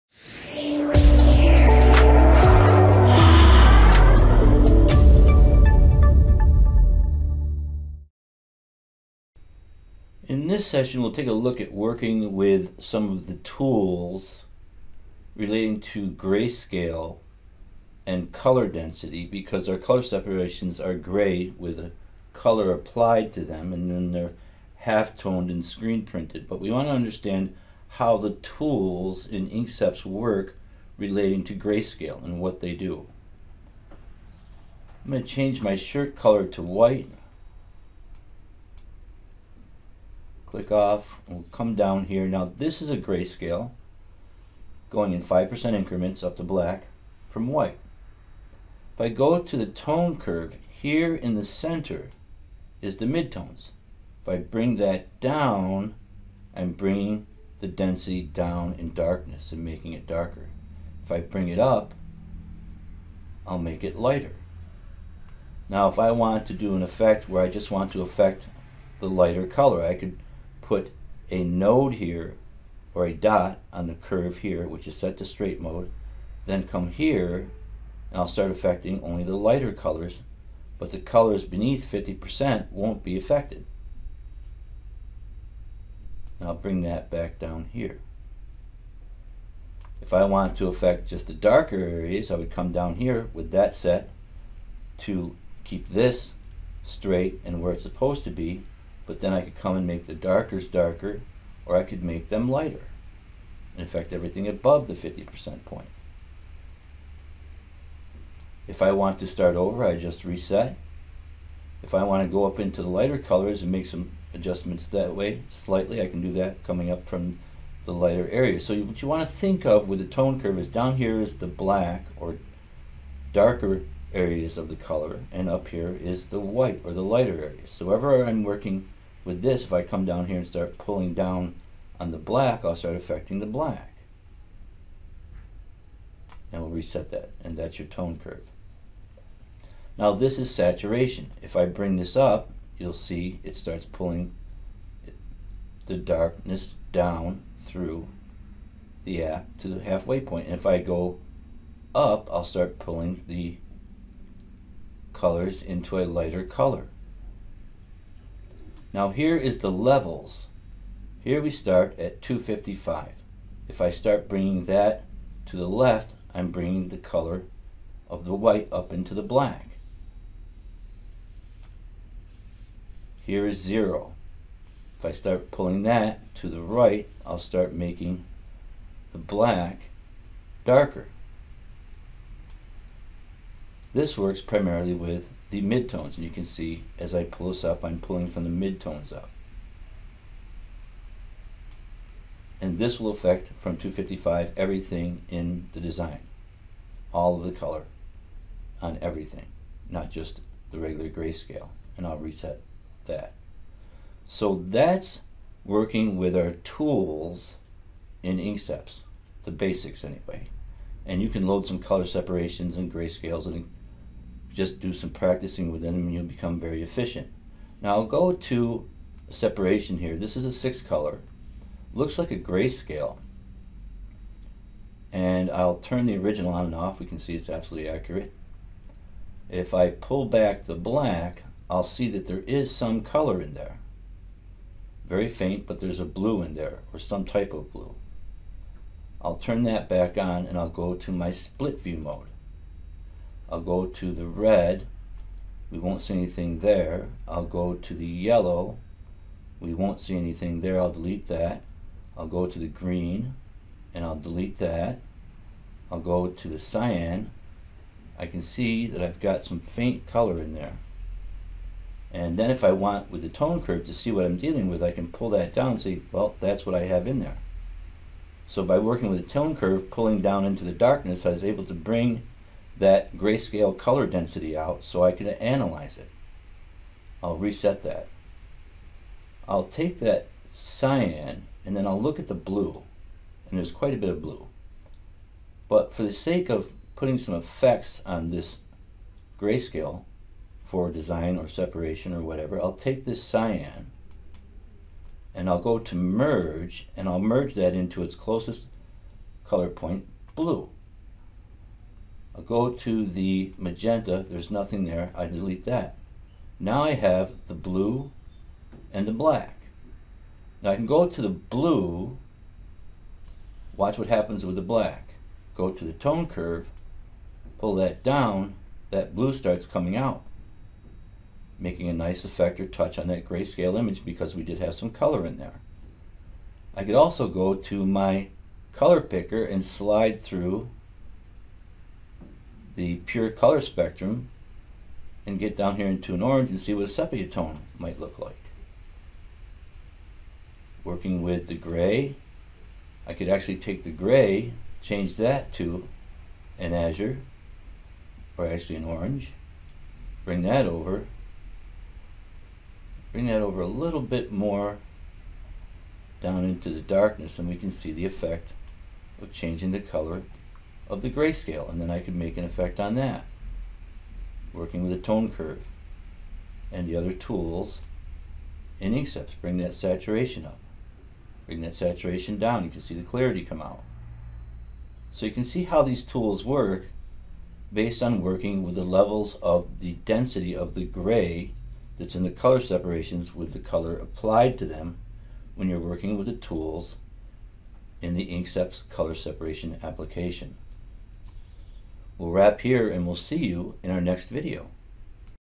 Working with the tools in the InkSeps Color Separation APP
In this video, we will look at working with the adjustment tools in the InkSeps simulated process color separation APP